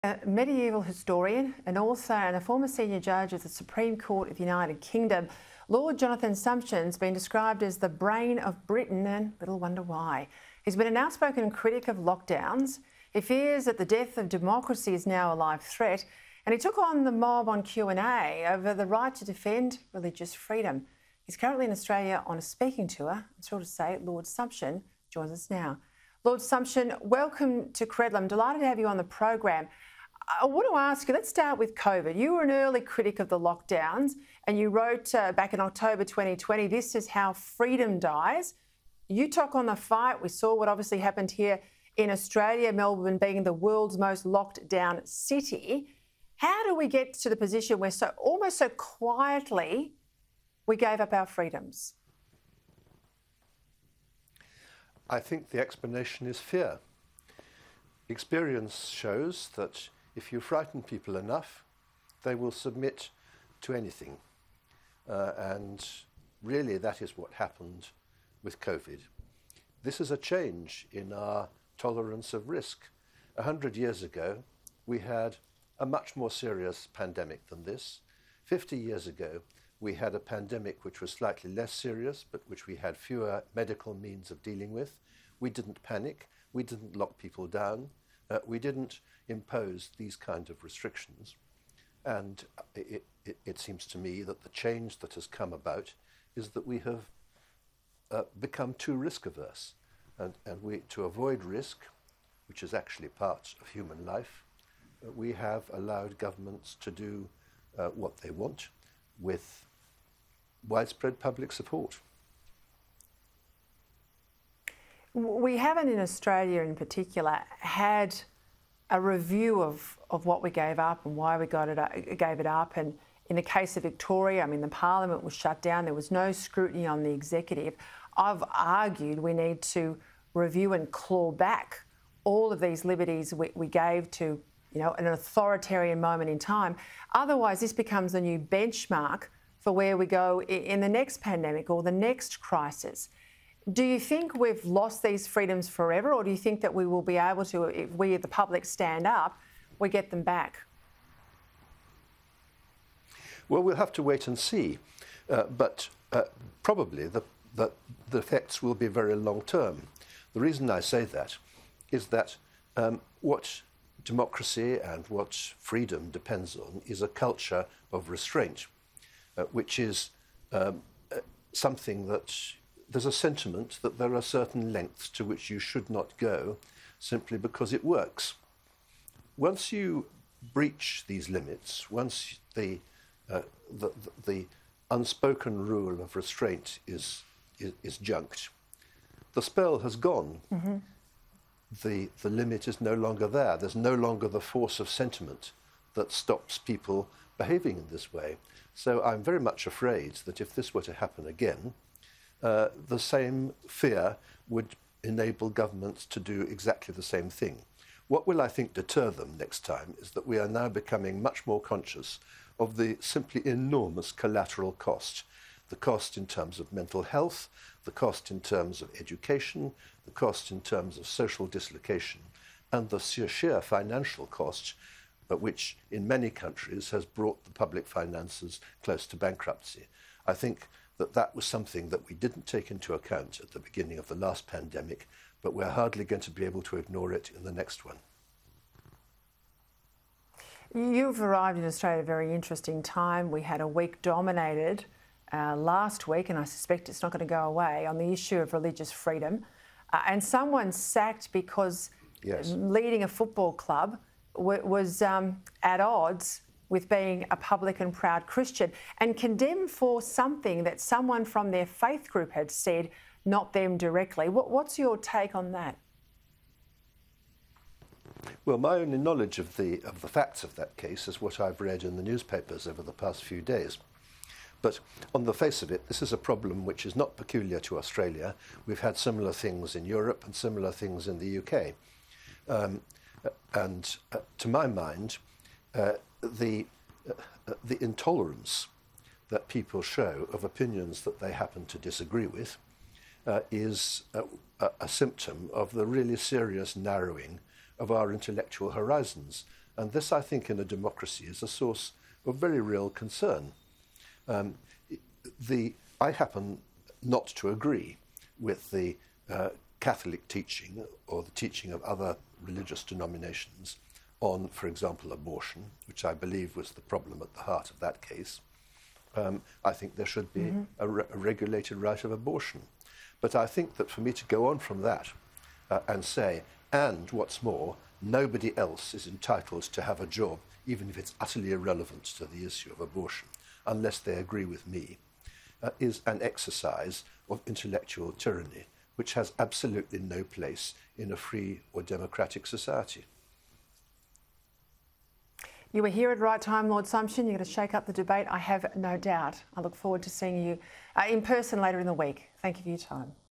Governments ‘frightened’ people during COVID-19
Former Justice of the UK Supreme Court Lord Jonathan Sumption says governments have been allowed to do “what they want” while still enjoying “widespread public support”.

“Experience shows, if you frighten people enough, people submit to anything. And really, that is what happened with COVID,” he said.